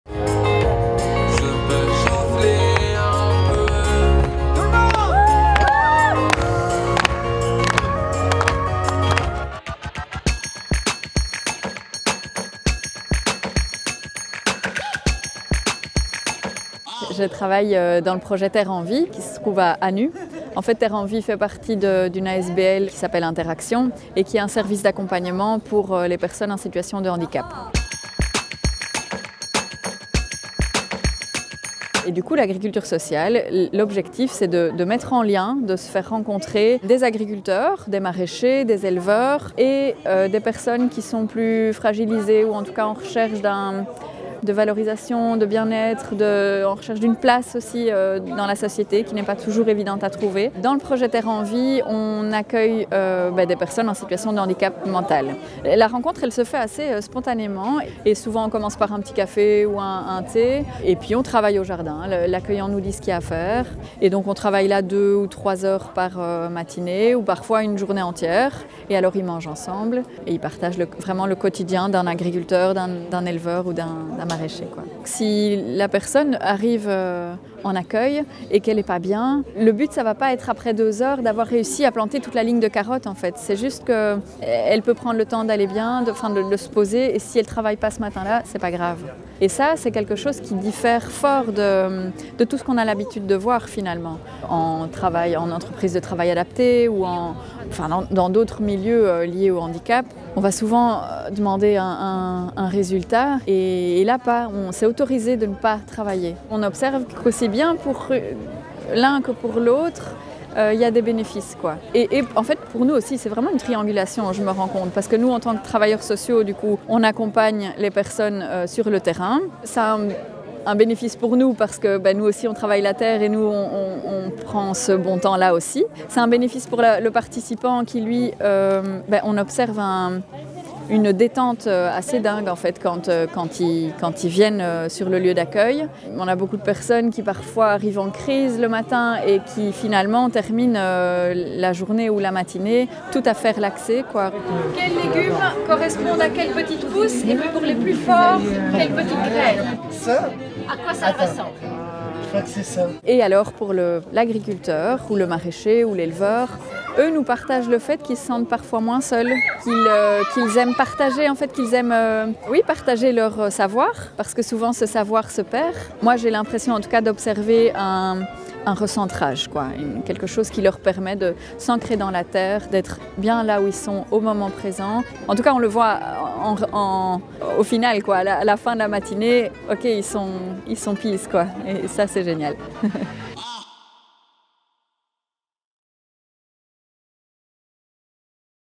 ICI#20 / FESTIVAL DE L’AGRICULTURE SOCIALE
Ce samedi, c’était le Festival de l'agriculture sociale à l'Abbaye de Floreffe. Une journée consacrée à faire connaitre mais aussi à fêter et célébrer l'agriculture sociale en Wallonie.
De nombreuses fermes pratiquent l’accueil social. « L'agriculture sociale, ce n’est pas vraiment une thérapie, plutôt une façon originale de se remettre en mouvement au contact du vivant ».
Les acteur·rice·s de l'agriculture sociale se sont donnés rendez-vous à l'Abbaye de Floreffe pour faire découvrir le concept et se retrouver dans une ambiance conviviale.  Une journée résolument familiale et festive.

Découverte du projet « Terre EnVie » , porté par l’asbl Inter-Actions qui crée du lien entre les personnes en situation de handicap et des maraîcher·ère·s, agriculteur·ice·s ou éleveur·euse·s durables et éthiques.